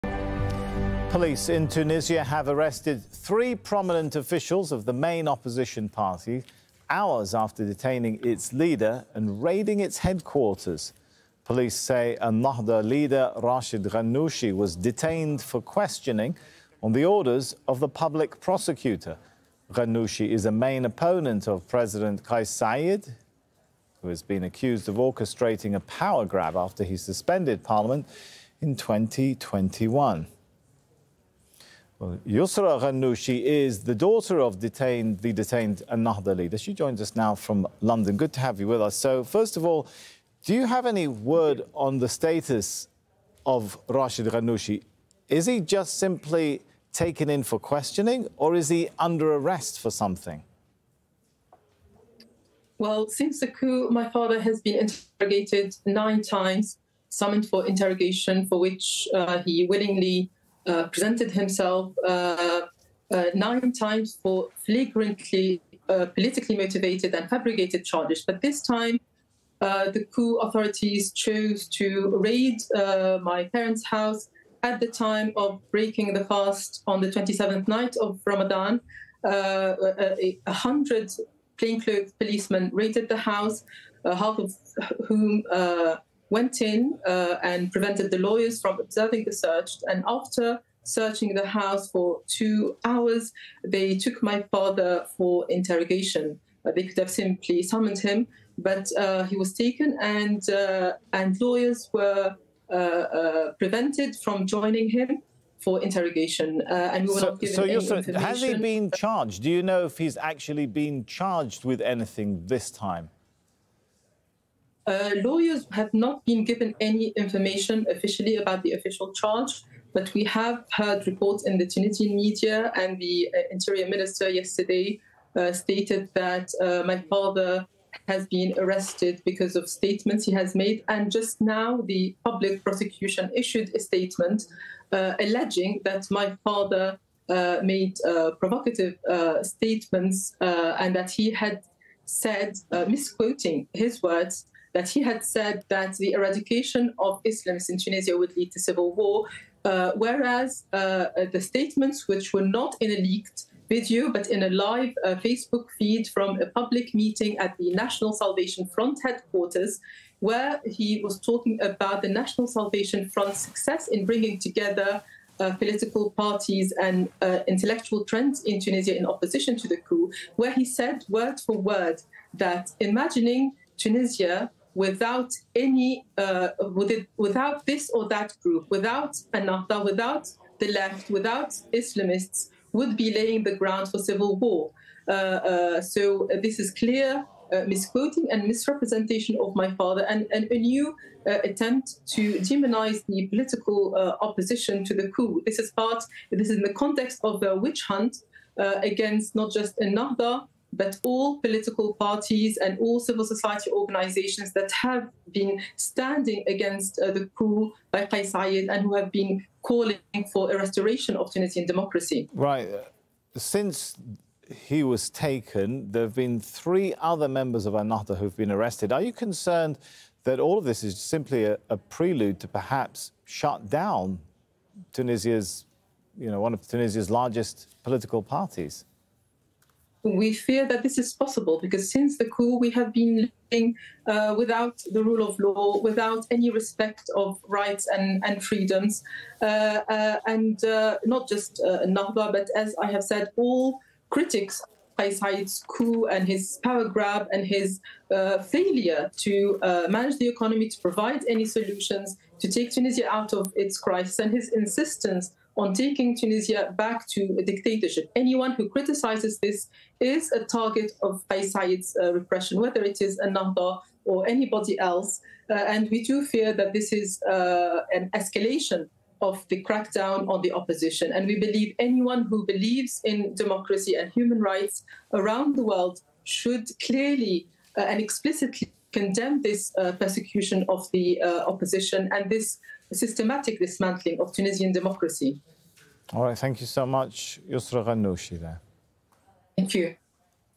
Tunisian police detain opposition leader, raid Ennahda party HQ
Tunisian police have detained opposition leader Rached Ghannouchi and raided the headquarters of his Ennahda party, party officials said.

Police raided Ghannouchi’s house on Monday evening, carrying out a search before taking him to what party officials called an “unknown destination”. Hours later, they began a raid on Ennahda’s headquarters.